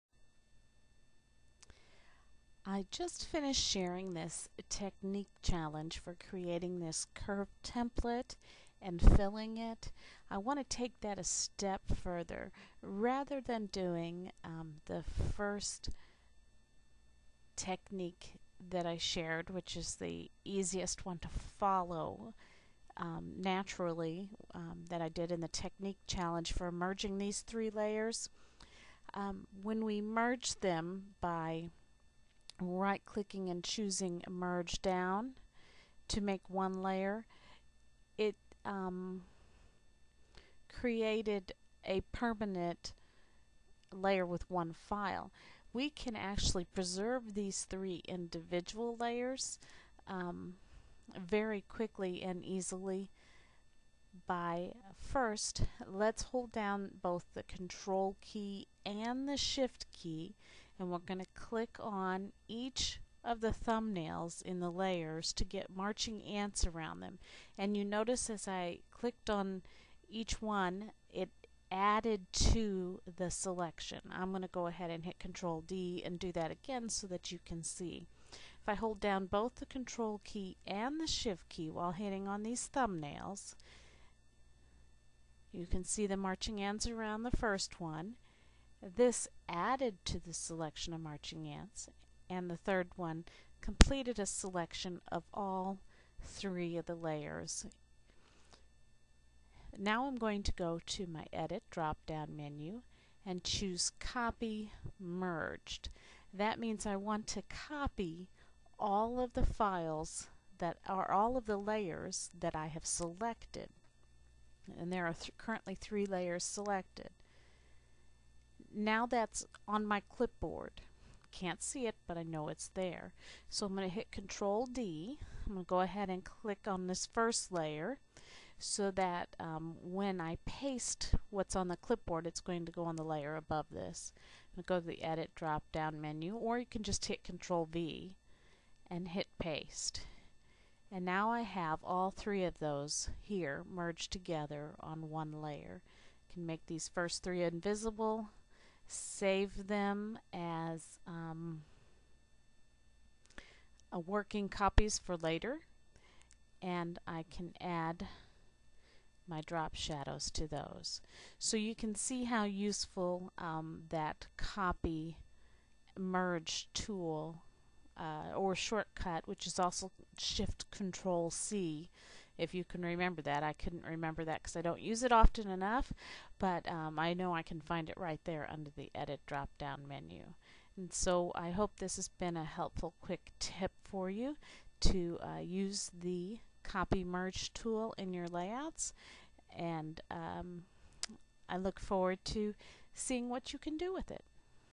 Quick Tip #9 Copy Merge
This is a quick tip  to follow my video for making curved photo templates in Photoshop Elements.  This is an alternative ending to the tutorial that can be applied in many other ways.